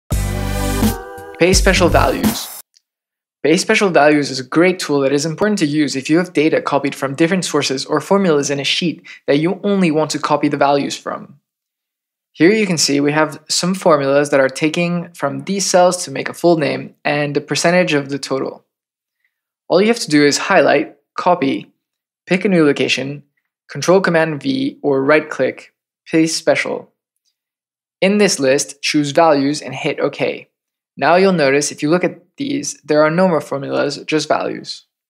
Excel Tips & Tricks: Paste Special Values
I am starting a new video series called Excel Tips & Tricks. Here is a video explaining the use of Removing Duplicate Valuese in Microsoft Excel. I am doing this because as an analyst I work daily on Microsoft Excel and therefore have some experience running into common issues and have some tips to go faster. I also need to improve my explaining/presentation skills so this is a great opportunity for me.